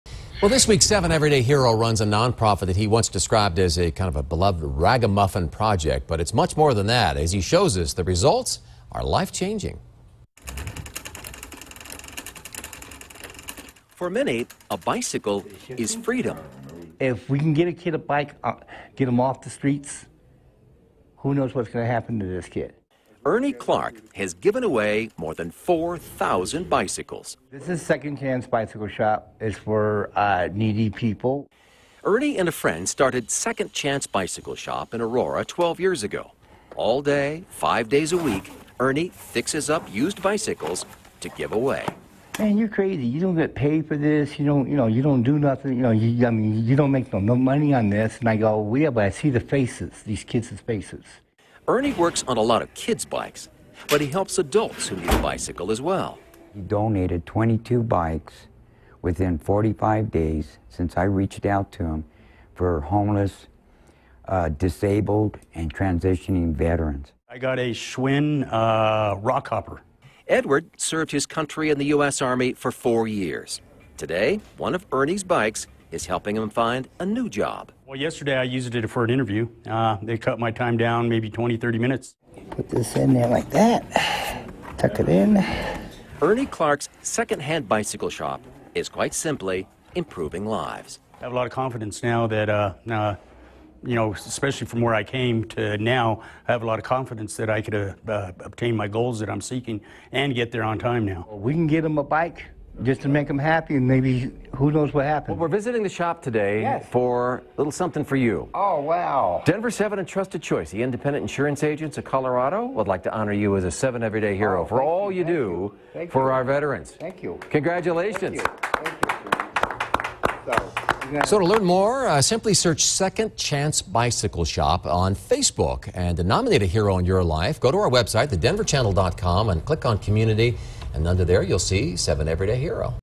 7Everyday Hero Ernie Clark
7Everyday Hero Ernie Clark repairs used bicycles at the Second Chance Bicycle Shop for those in need